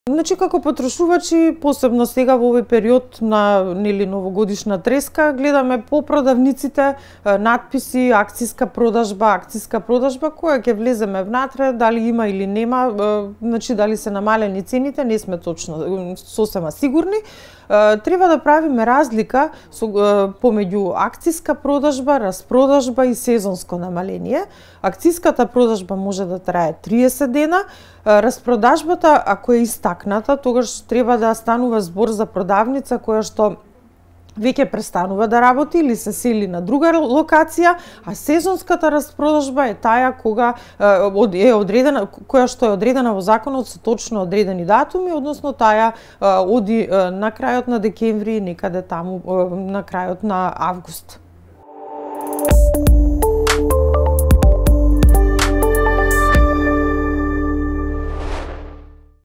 Која е разликата помеѓу распродажба, сезонско намалување и акциска продажба?
Одговара Татјана Тасевска од Организацијата на потрошувачите на Македонија.